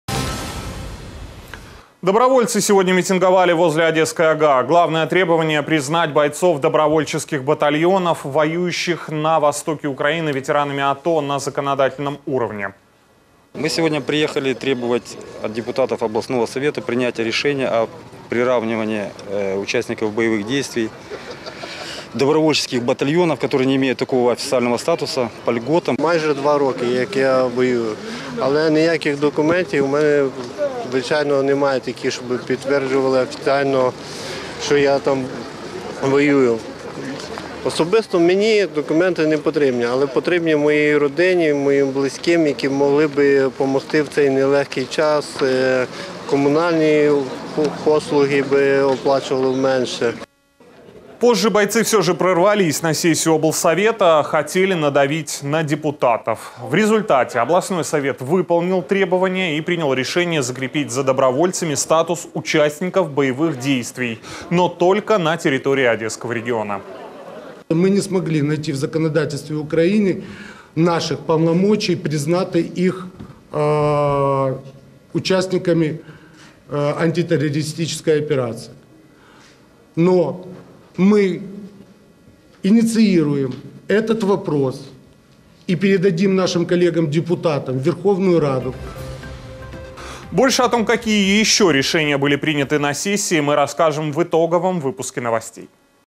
Митинг добровольцев под Одесской ОГА (обновлено)
Добровольцы сегодня митинговали возле Одесской ОГА. Главное их требование - признать бойцов добровольных батальонов, воюющих на востоке Украины, участниками боевых действий на законодательном уровне.

Одни из митингующих отметил:

    «Почти два года я воюю, однако никаких документов у меня конечно нет, которые подтверждали бы официально, что я там воюю. Лично мне документы не нужны, нужны моей семье, моим близким, им могли помочь в это нелегкое время, коммунальные услуги платить меньше».

По словам активистов, этот статус даст и право получать боекомплекты, форму, а также питание - все, что нужно в условиях боевых действий. Примерно через час после начала сессии облсовета бойцы прорвались на заседание.

В результате областной совет выполнил требование митингующих и принял решение закрепить за добровольцами статус участников боевых действий, но только на территории региона.